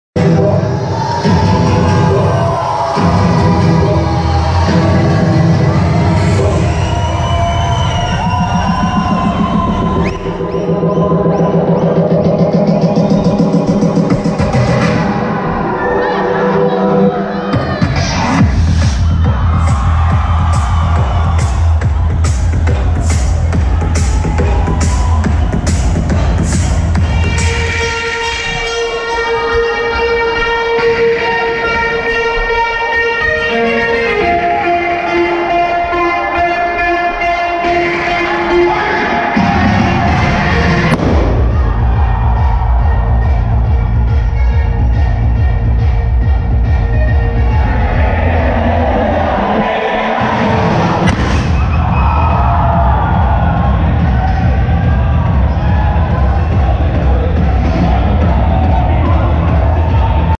ABDUÇÃO NA FÁBRICA
ABDUÇÃO REALIZADA COM SUCESSO ! Show que entrou para a história do nosso projeto. Warm-up da festa Progressivamente, na Fábrica Criativa em Belo Horizonte, Minas Gerais, Brazil.